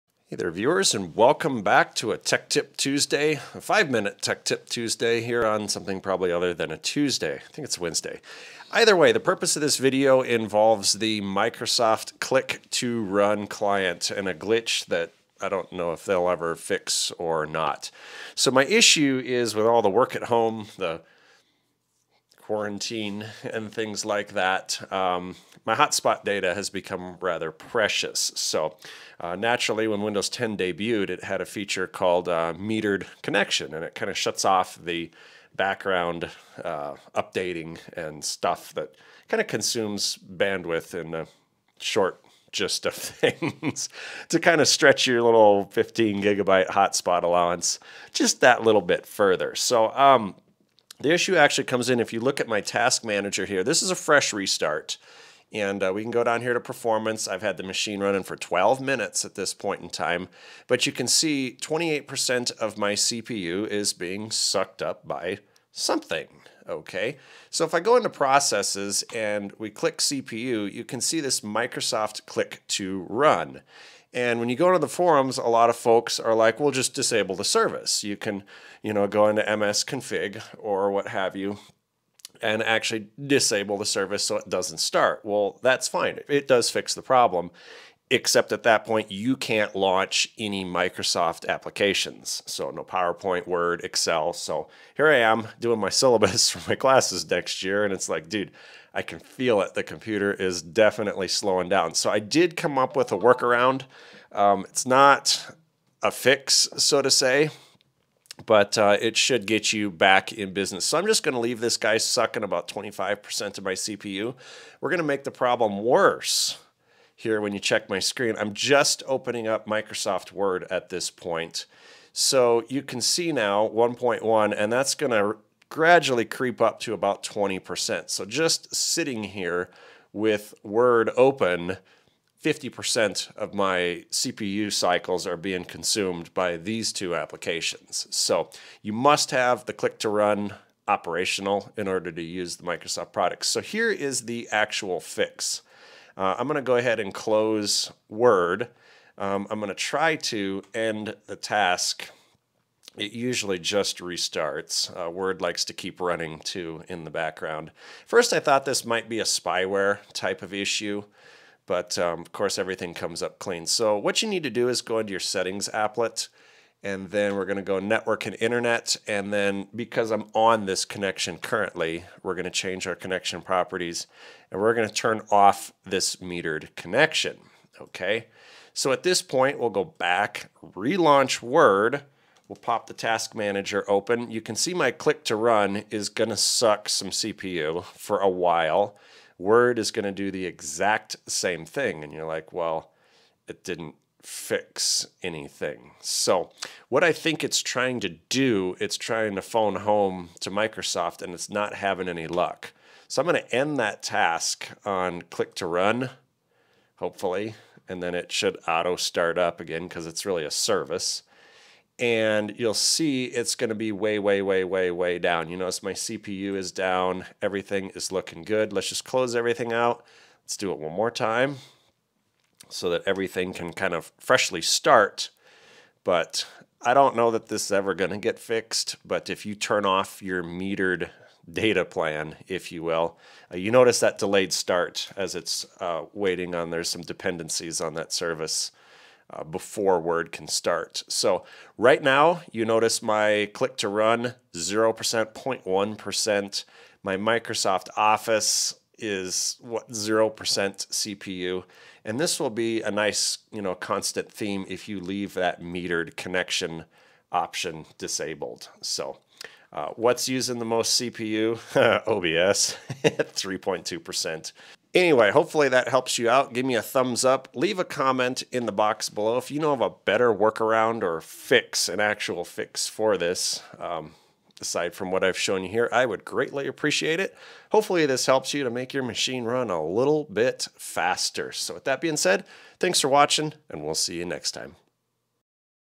Microsoft Office Click to Run Service Excessive High CPU Usage Microsoft Word 2019, 2016
TL;DR - Disable metered connection.
Join us for this Tech Tip Tuesday where we work around an issue with Microsoft Office Click to Run Service consuming excessive CPU.  This issue seems to have surfaced after an update to Office and can really dog the system down even if you don't have Office open.

We are running Office Professional Plus 2016.